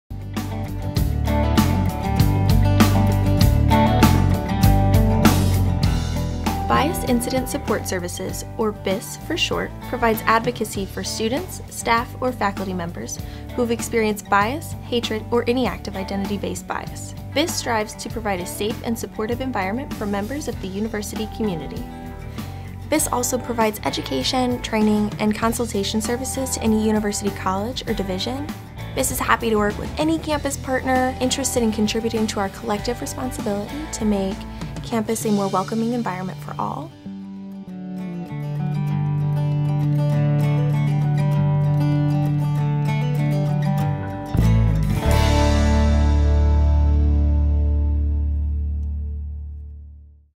Well Kentucky Resources: Bias Incident Support Services (BISS)
Learn more about Bias Incident Support Services (BISS) at the University of Kentucky.